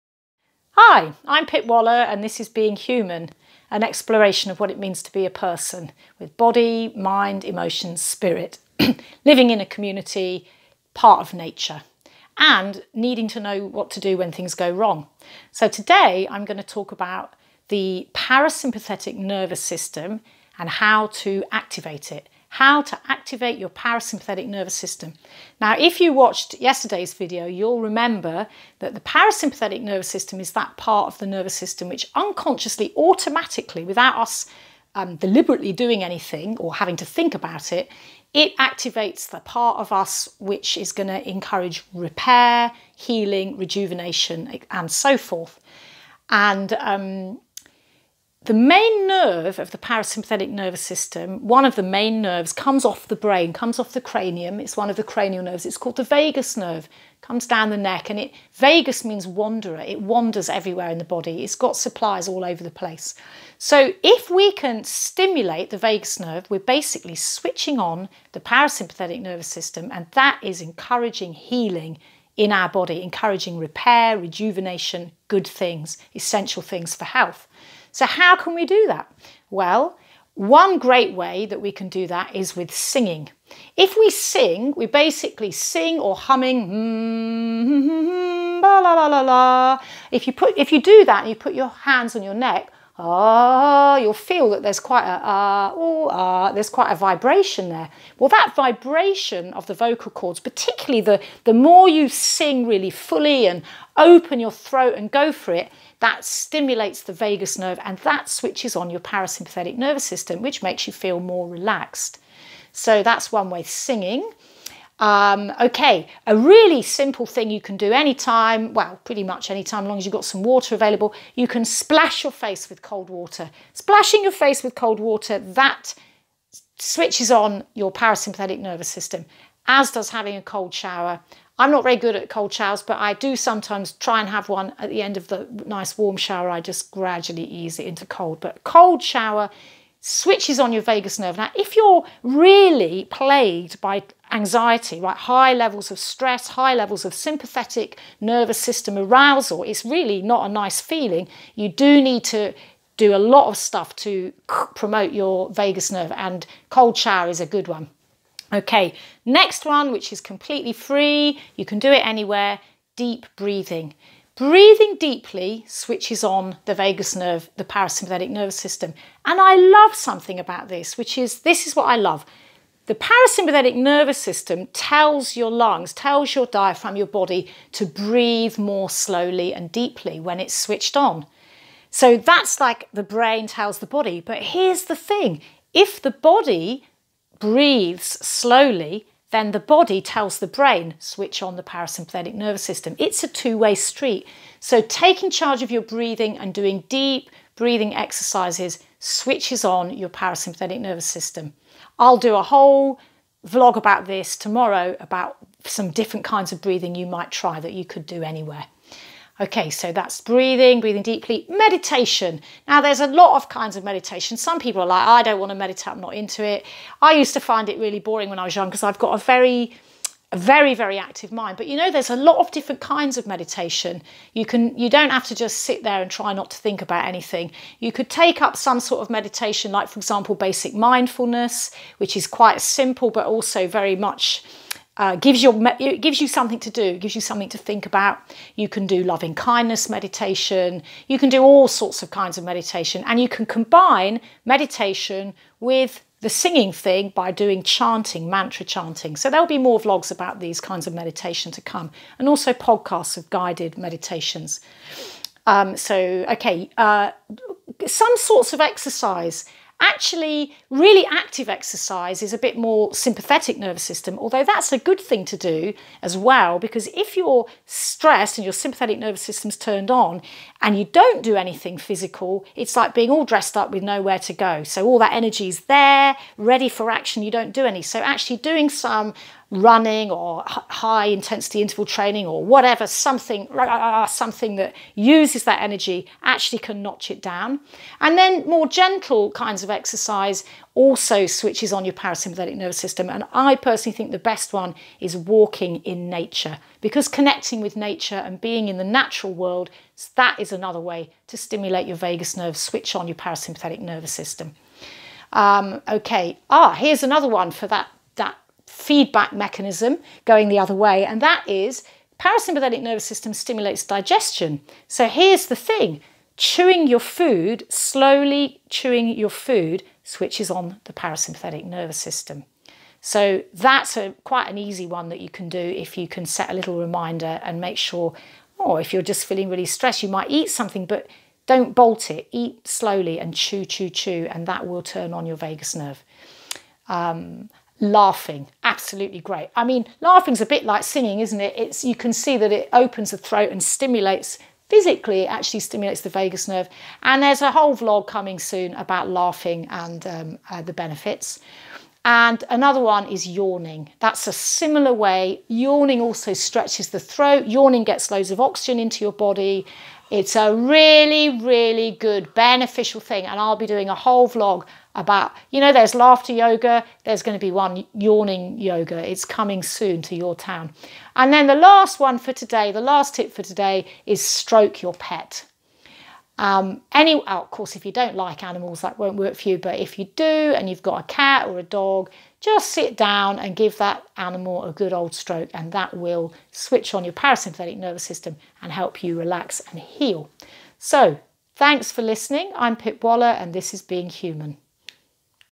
Vagus nerve turn-ons
The vagus nerve  is a major nerve of the parasympathetic nervous system.
Healing, regeneration, rejuvenation, repair, recovery and refilling the body's reserves happens when the autonomic nervous system is in parasympathetic mode. This short video describes some easy ways to stimulate the vagus nerve and so switch on  this powerful mode more fully.

You can buy my book Deeply Holistic- a guide to intuitive self-care, as well as my other books, from my